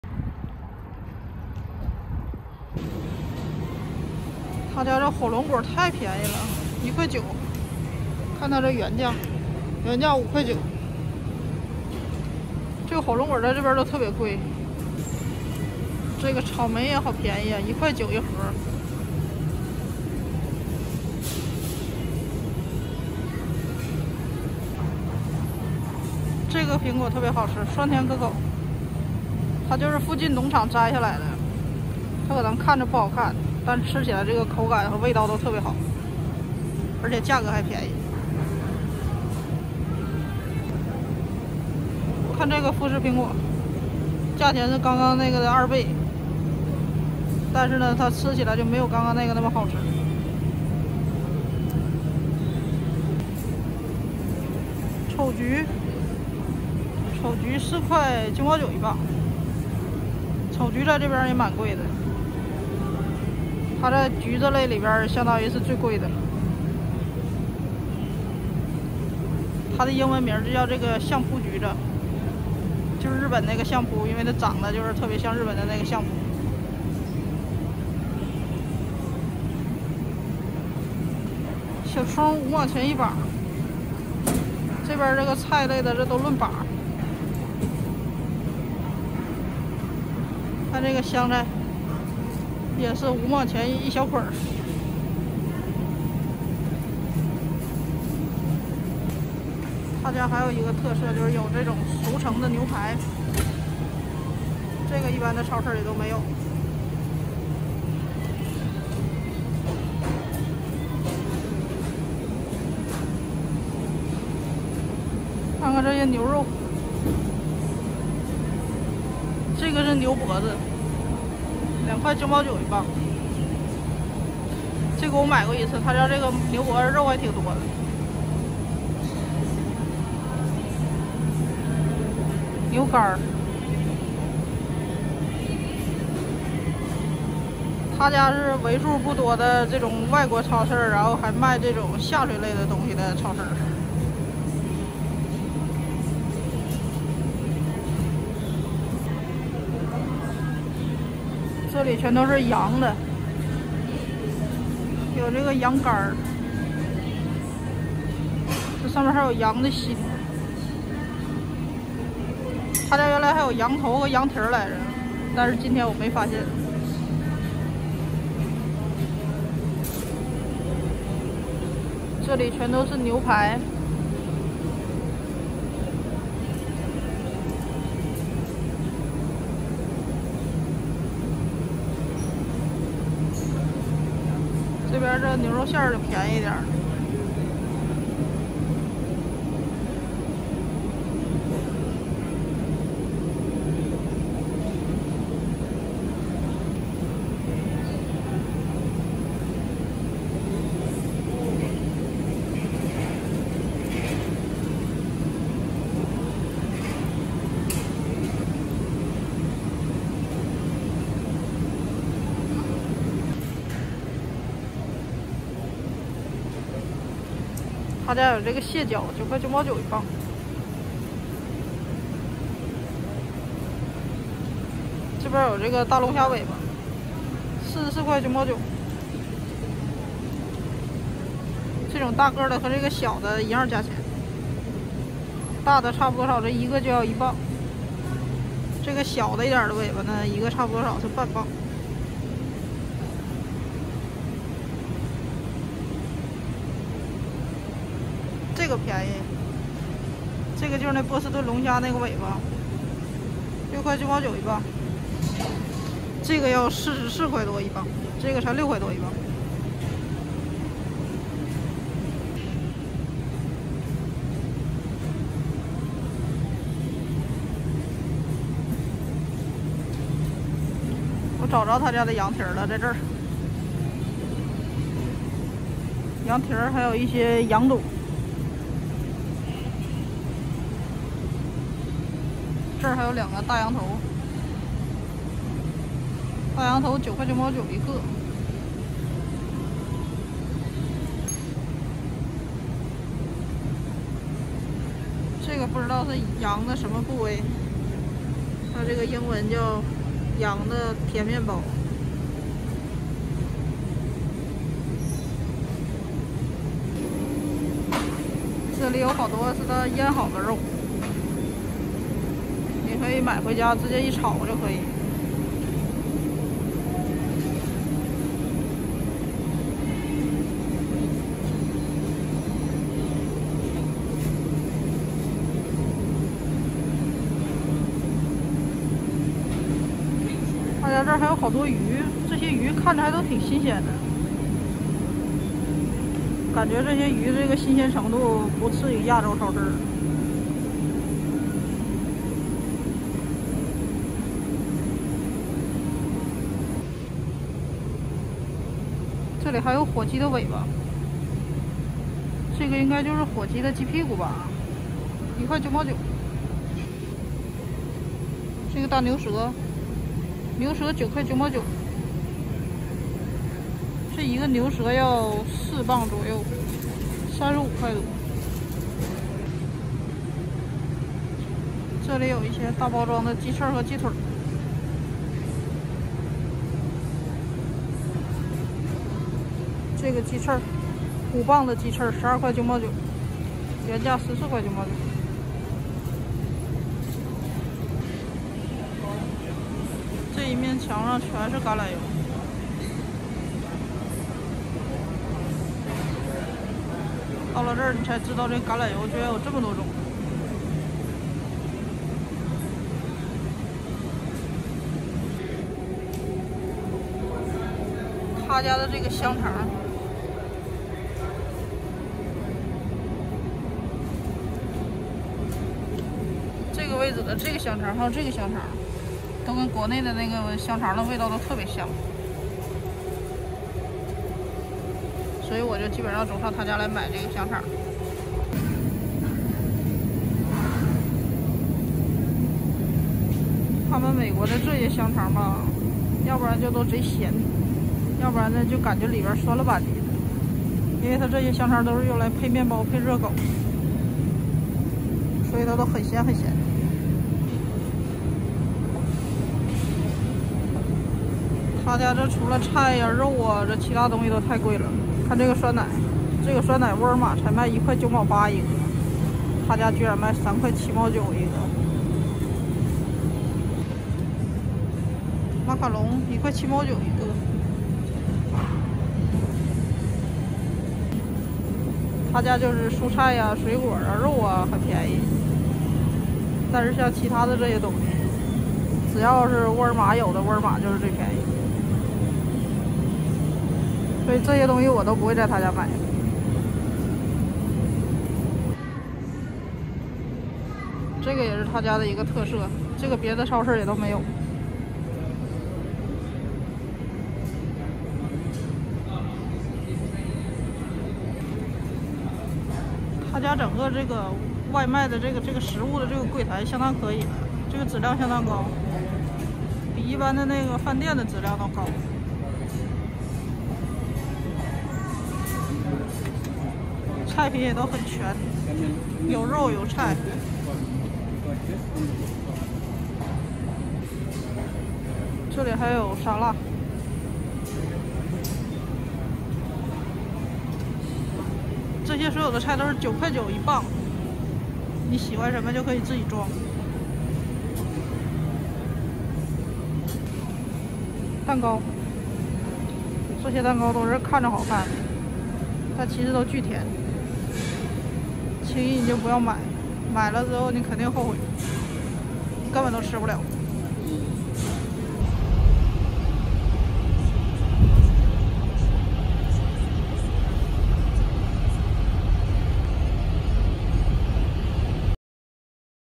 逛逛超市，看看美国物价，一家带外卖的超市都会买点什么？
了解美国物价
看看美国的百姓食品